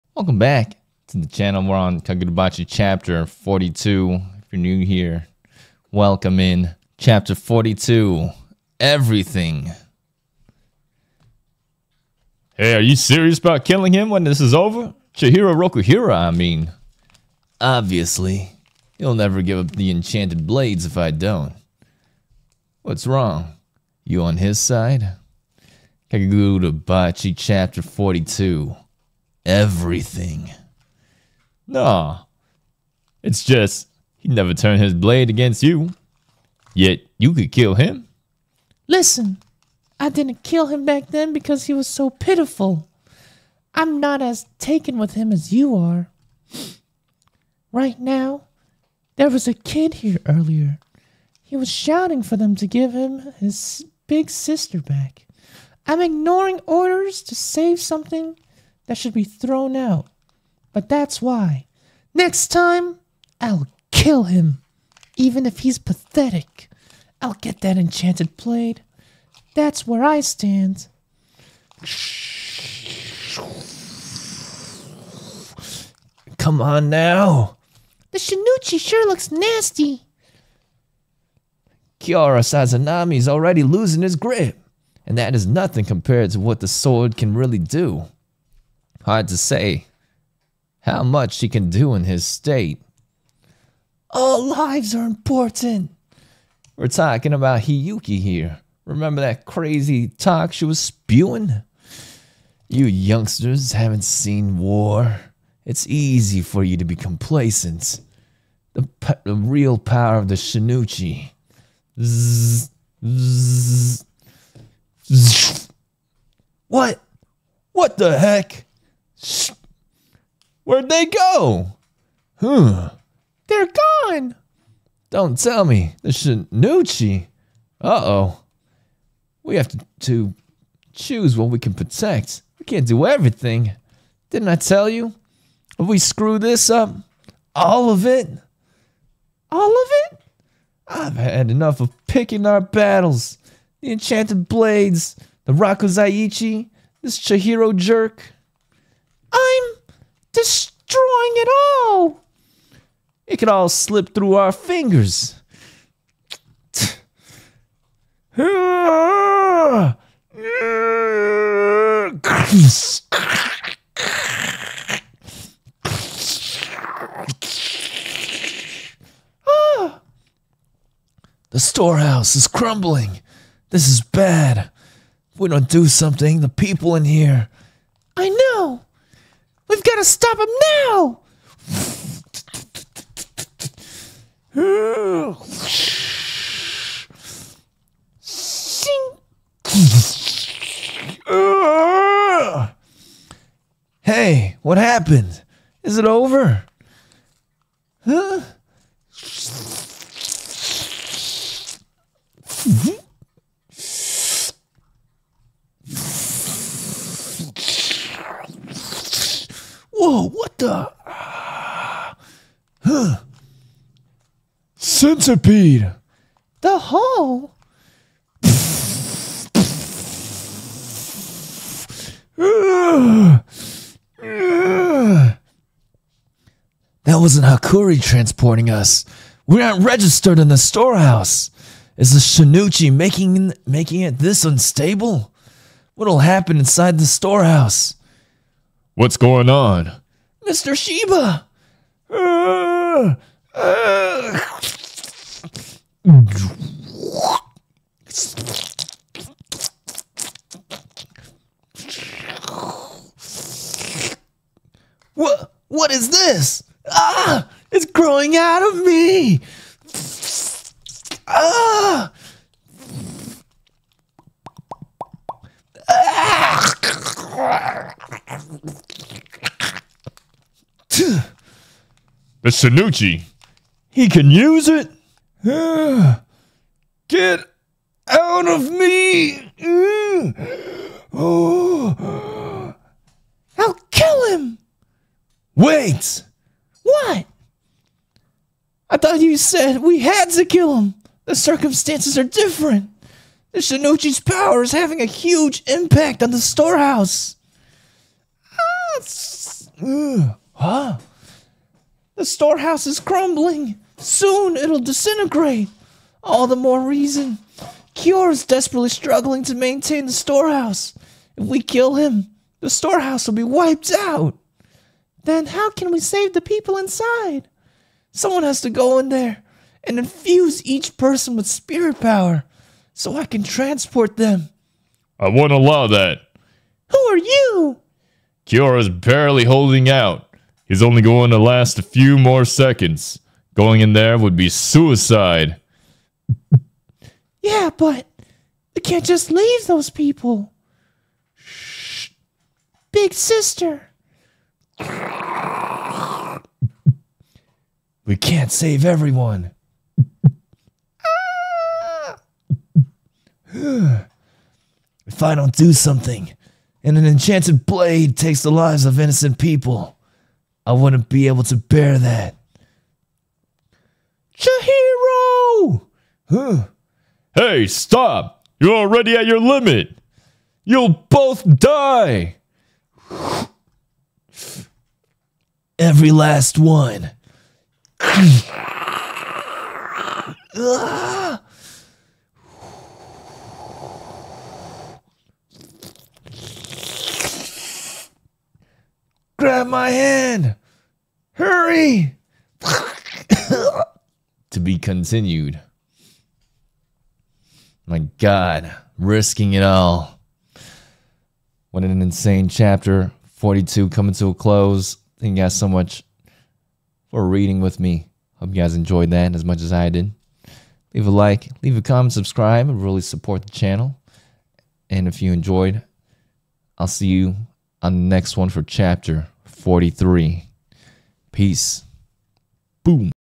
Everything
Kagurabachi Chapter 42 Everything

Kagurabachi is about Chihiro Rokuhira, the son of a renowned blacksmith who forged six enchanted swords, seeks a bloody revenge against a gang of sorcerers with the help of a seventh enchanted sword forged by his father before he was assassinated.

⦊ EPIC CODE: KIKIKATME

Live Monday - Friday 5PM PST